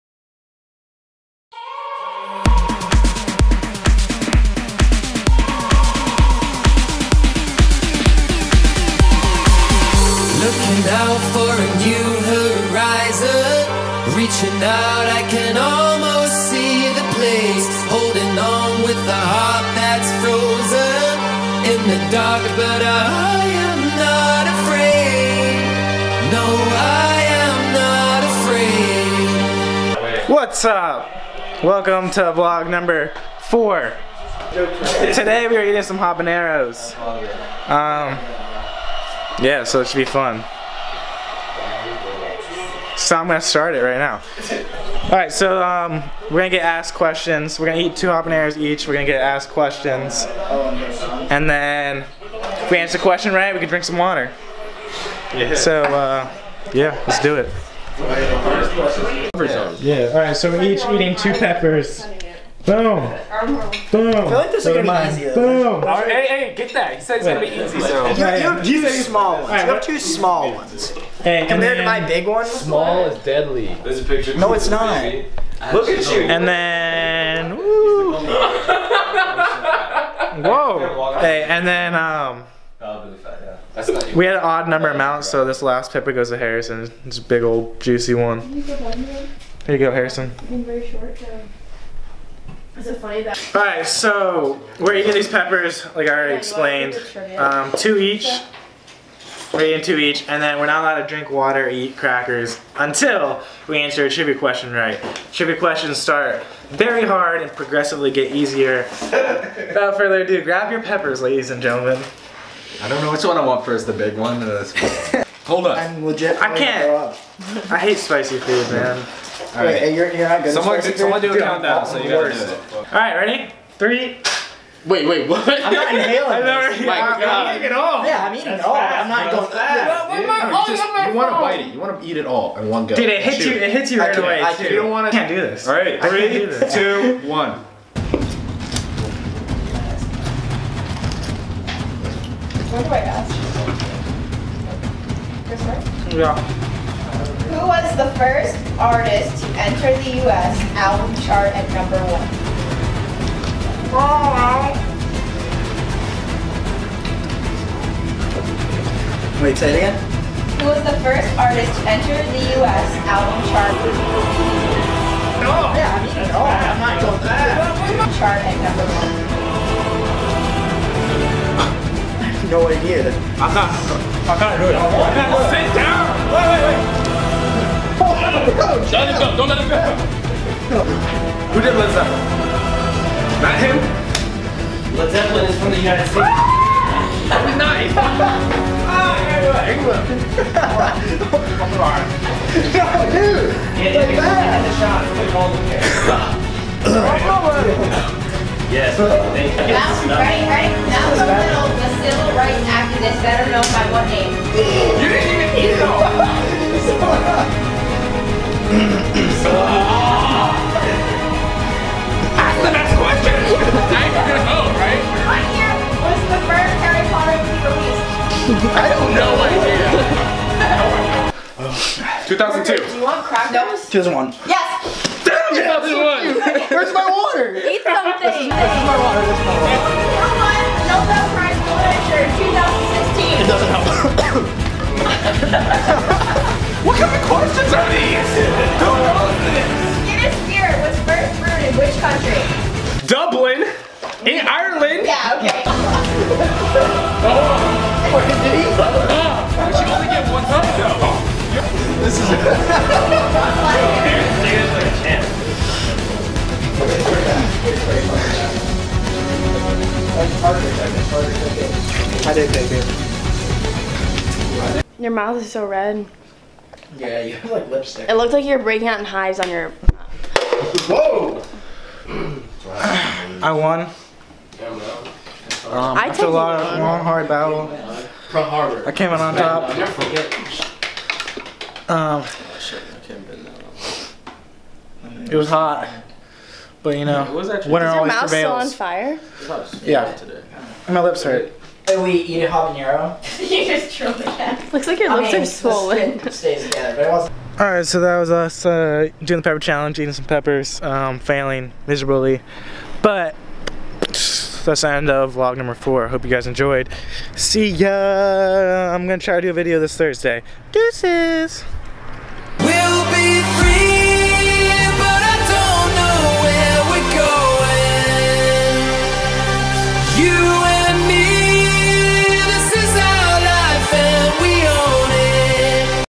Eating Habanero Peppers! (do not try this) - vlog 004
Let's answer some trivia questions and eat some peppers